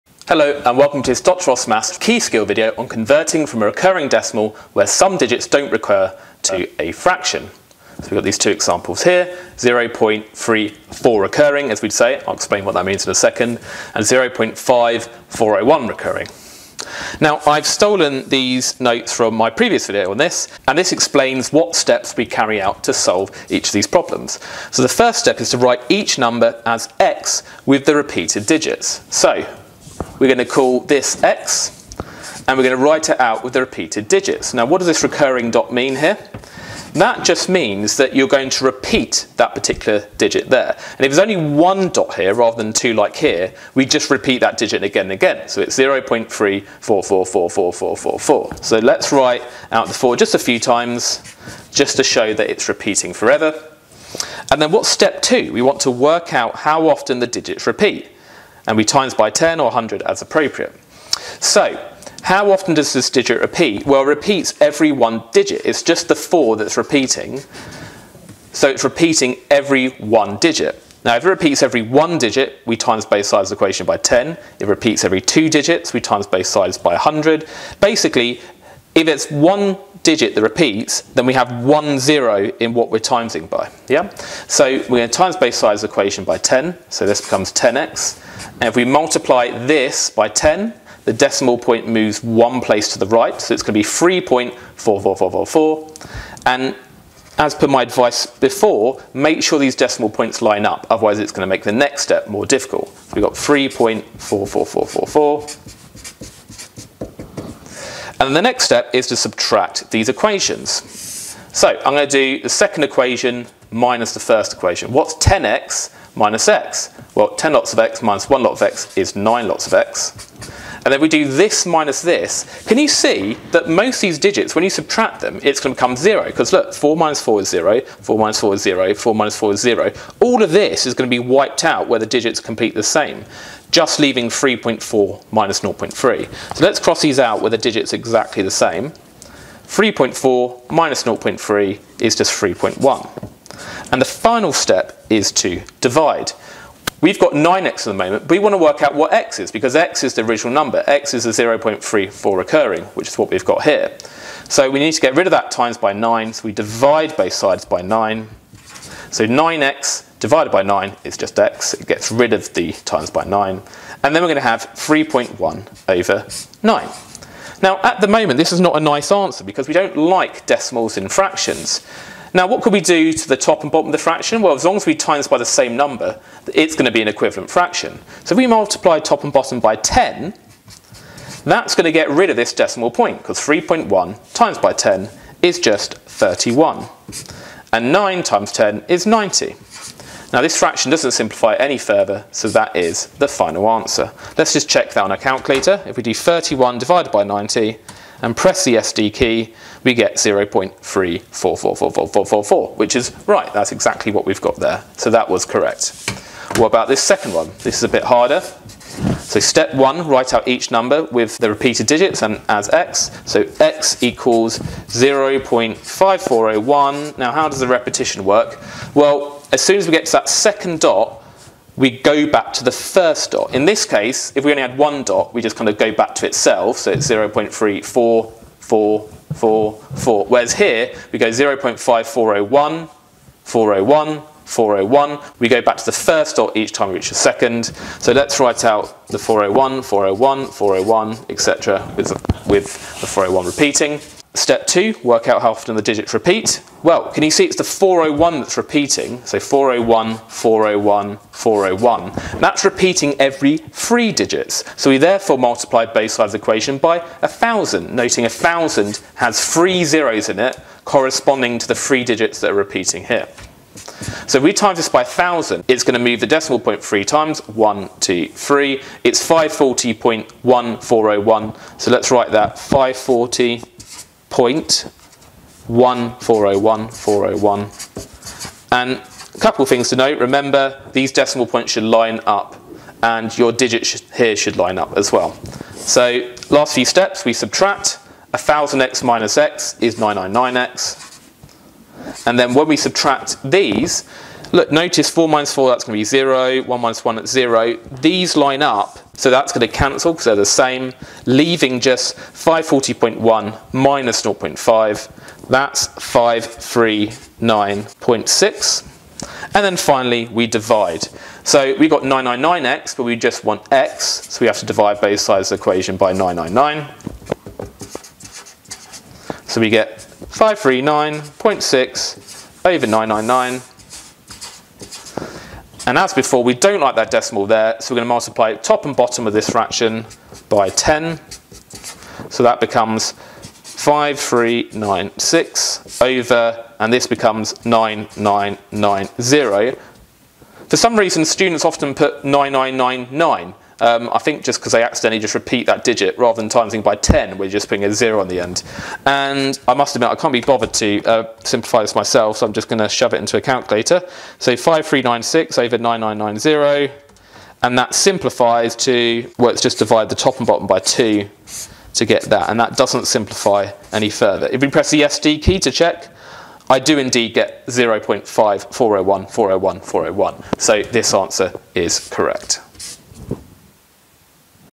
Key Skill - Convert from a recurring decimal to a fraction where some digits don't recur.
"Convert from a recurring decimal to a fraction where some digits don't recur."